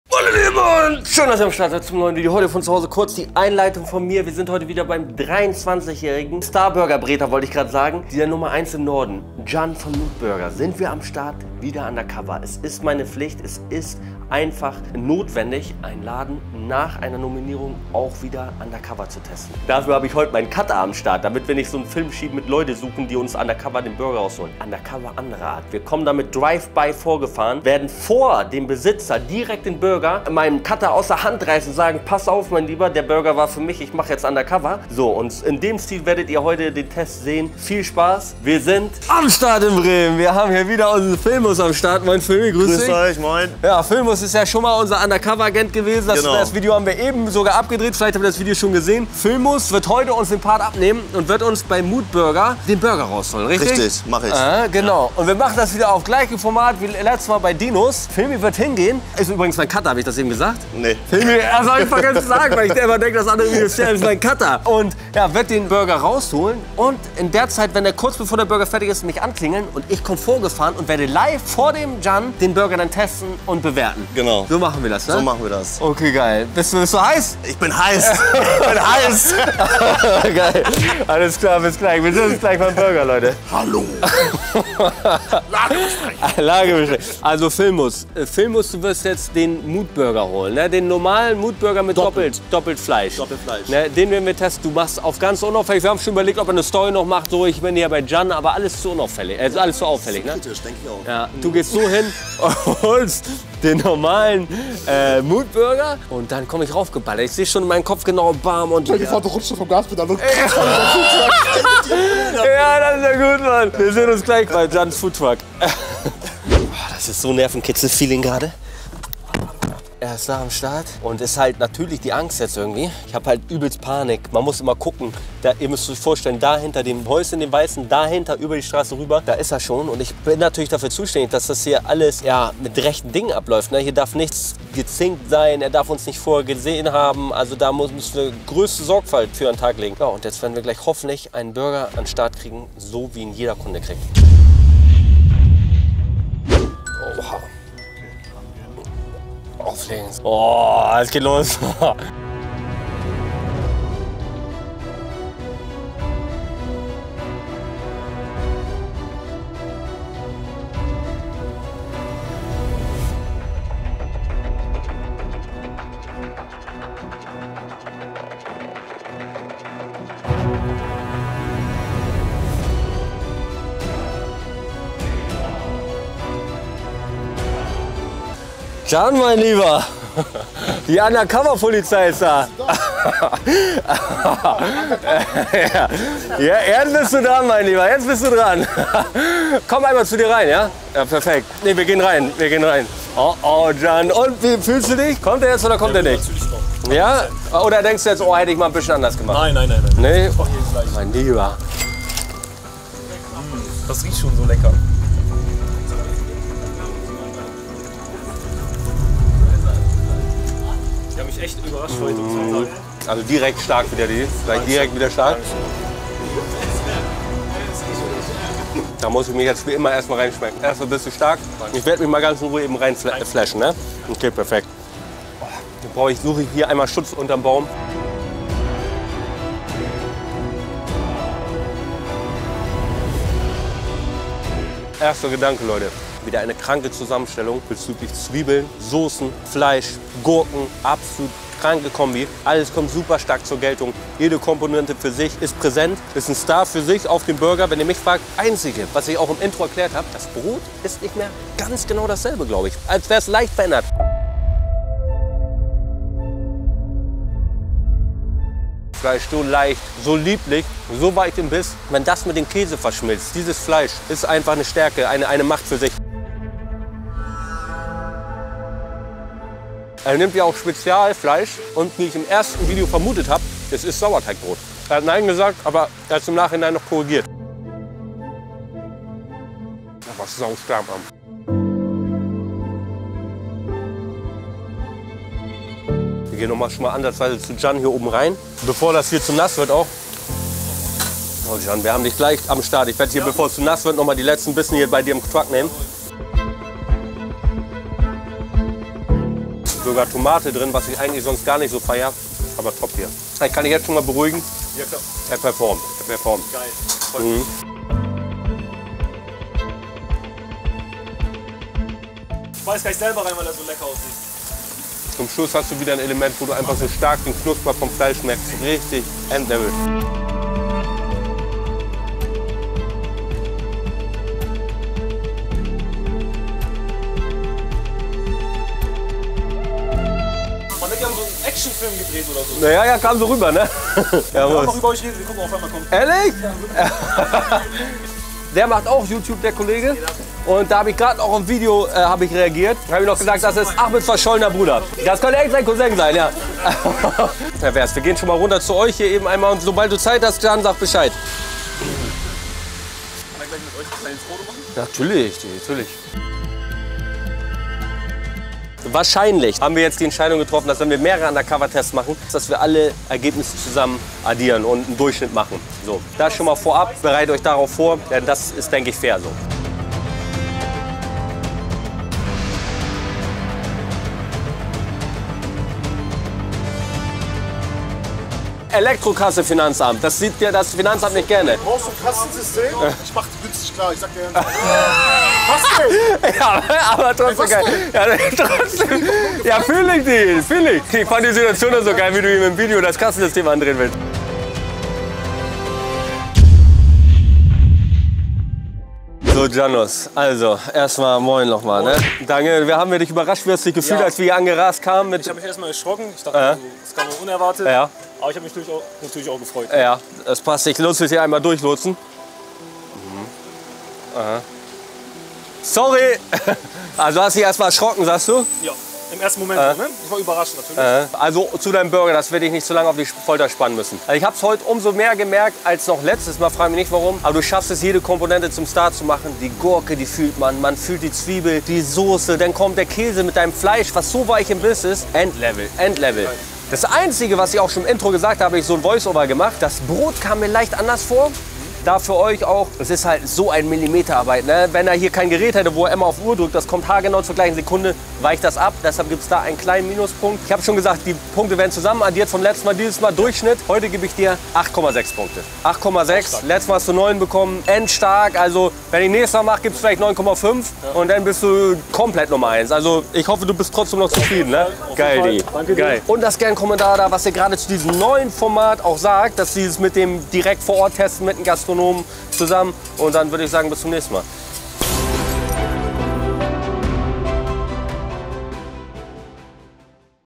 UNDERCOVER beim BESTEN BURGER im NORDEN!
Den besten Burger im Norden findet man bei Mood-Burger in Bremen. Der 23 jaährige konnte sich glatte 9/10 Punkten erarbeiten und bietet definitiv einen Burger an, den du in dieser Art nirgendwo anders finden wirst. Heuete bekommt er mein neues Undercover-Format zu spüren!

Meine Produkte:
Saftig-Shop.de
Code: Holle10
Adresse: Neuenlander Str. 37, 28199 Bremen
Gute Arbeiter:
Feinschnitt, Finales Video: Filmjohann